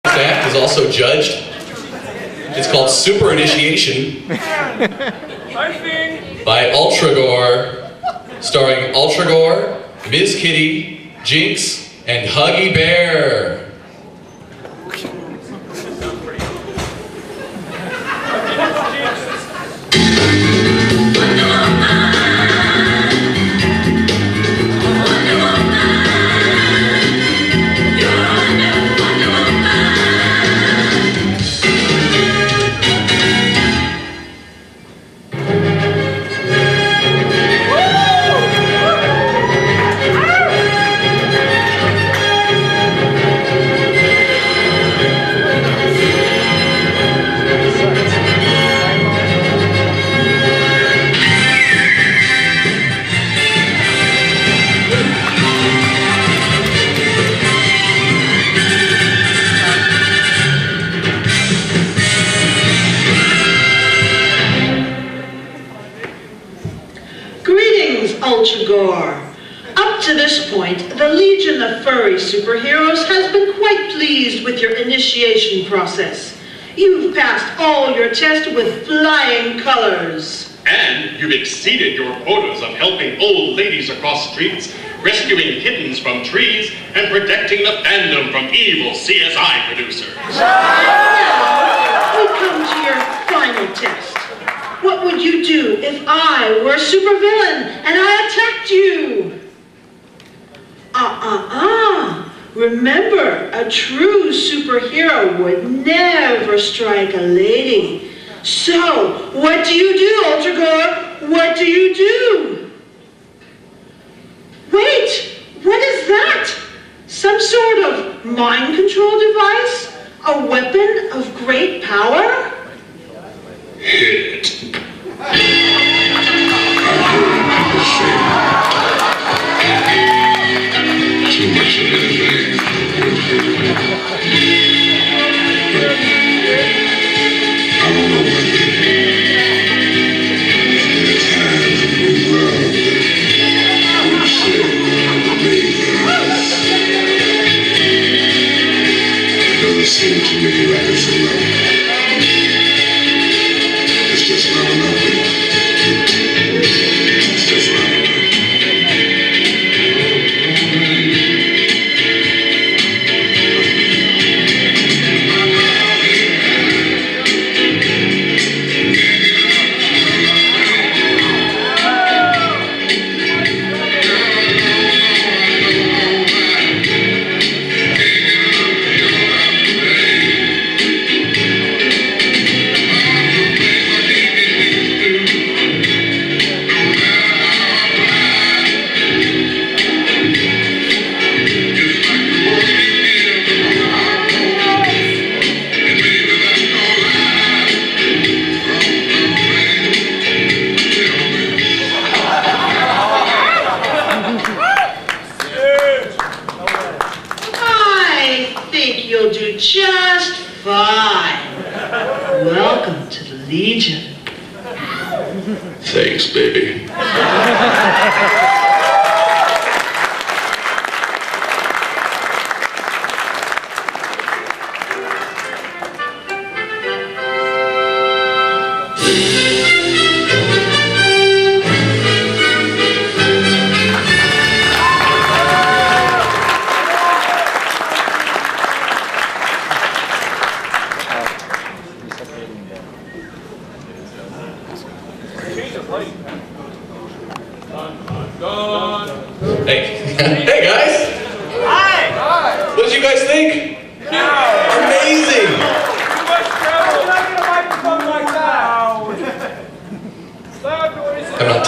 Furry Night Live '06--"Super Initiation"
furry costume convention cosplay spandex kitty bear mouse barrywhite sing muscle trunks Legion of Furry Superheroes must test their newest inductee: Ultra-Gor, gorilla superhero!